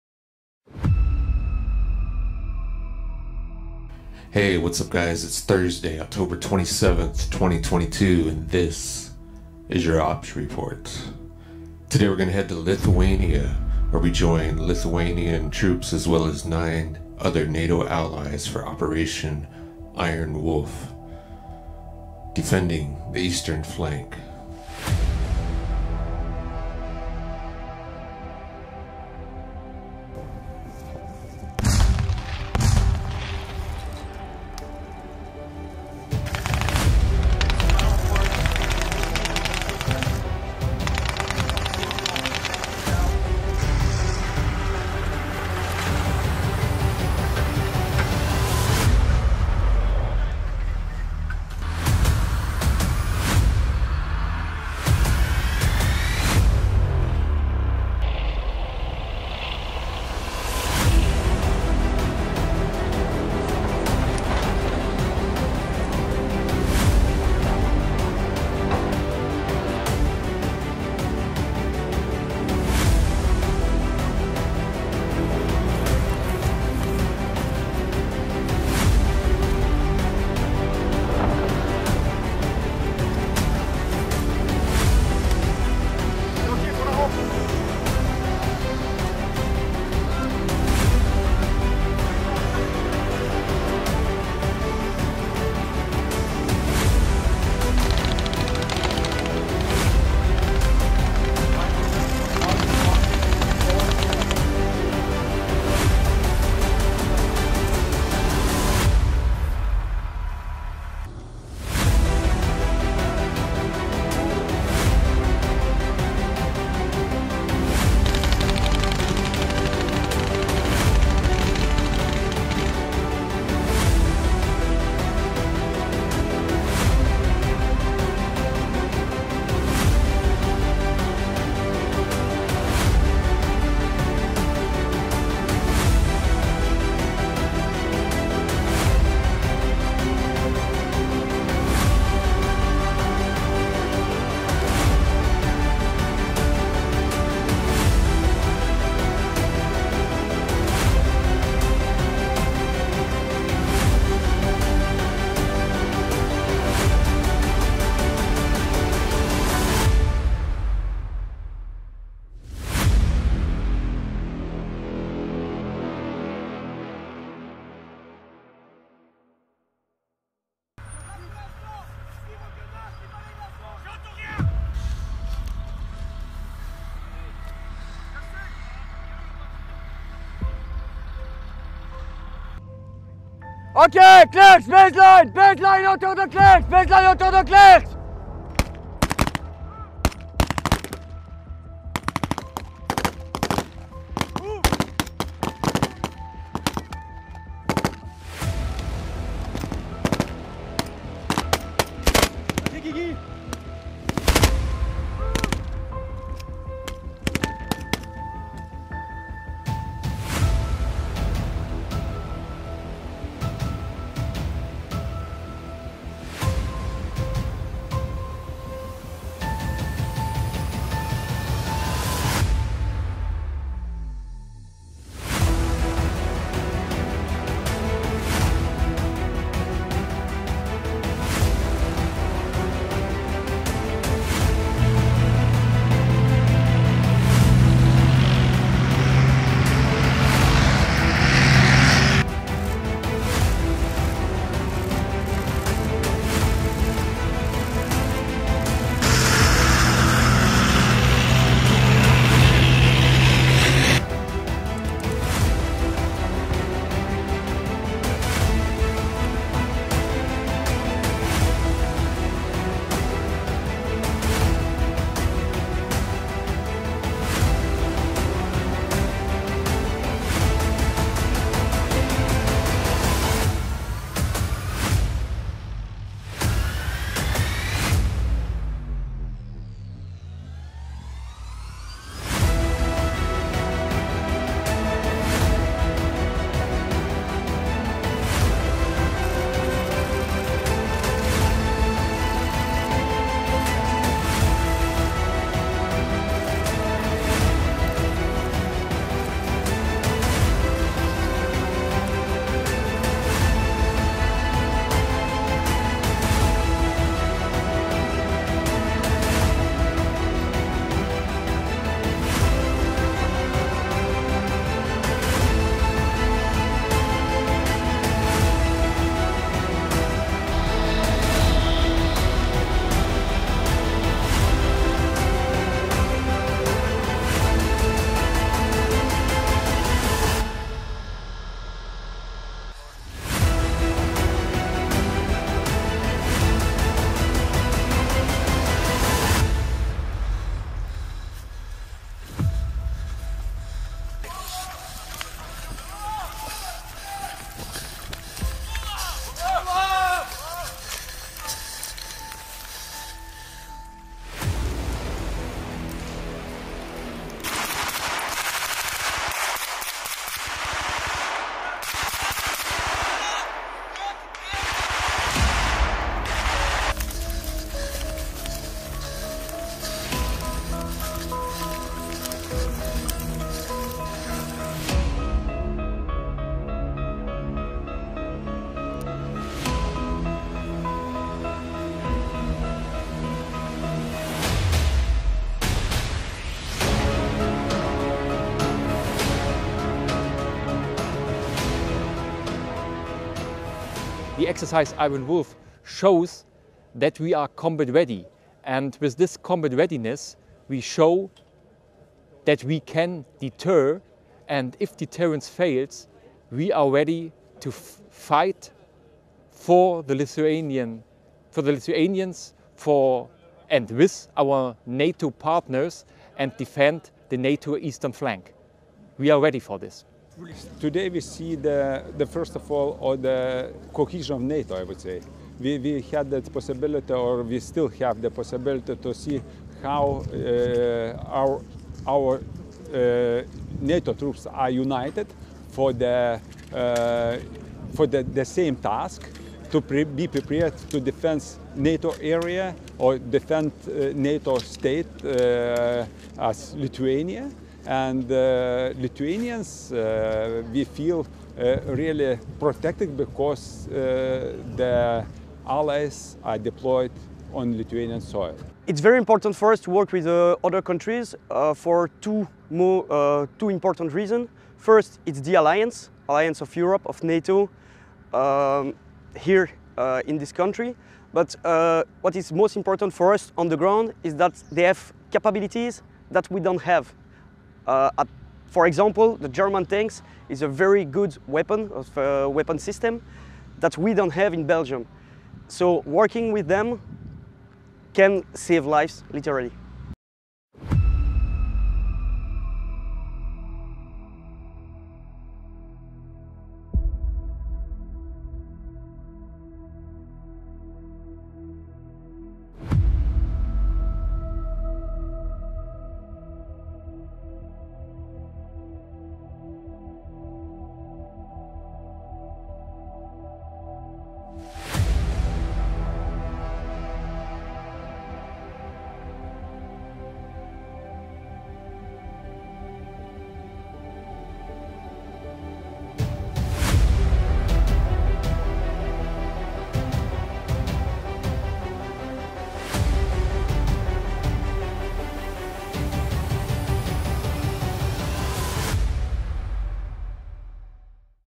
OPS REPORT: Lithuania and NATO Allies prepare for WAR on Eastern Flank
In todays OPS REPORT, we join Lithuanian forces as well as forces from eight other NATO allies, including Belgium, Czechia, Germany, Luxembourg, the Netherlands, Norway, the United Kingdom and the United States in Operation Iron Wolf.

The operation involves more than 3,500 troops, and tests the ability of the Lithuanian Army’s Iron Wolf Brigade to carry out defensive operations and counter-attacks. NATO’s German-led multinational battlegroup based in Lithuania is participating, as is a US Army battalion assigned to Lithuania on a rotational basis.

THE ALLIANCE IS STRONG!